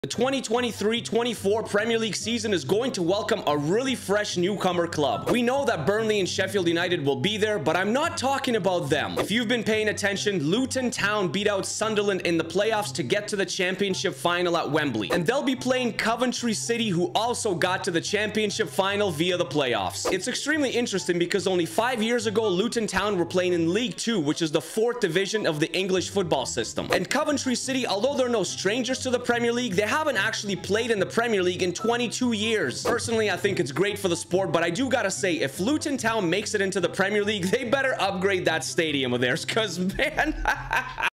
A FRESH PREMIER LEAGUE CLUB IS COMING
LUton Town and Coventry City will battle it out for a spot in the Premier League next season! Who will come out on top?

 priority on future transfers in any series!